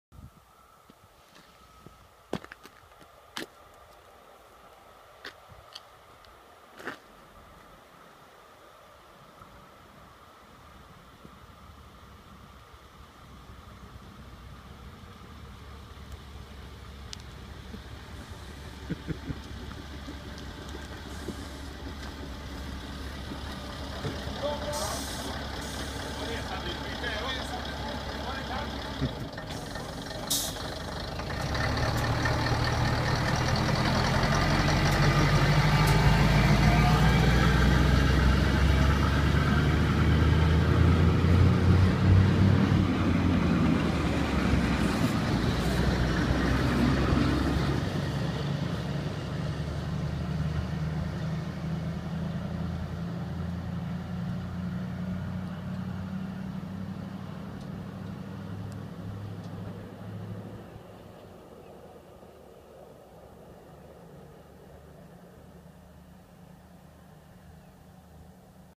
SMoK i Hörle
Sv. Motorvagnsklubbens tjänstetåg från Värnamo till Hultsfred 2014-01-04.
Påstigande i Hörle.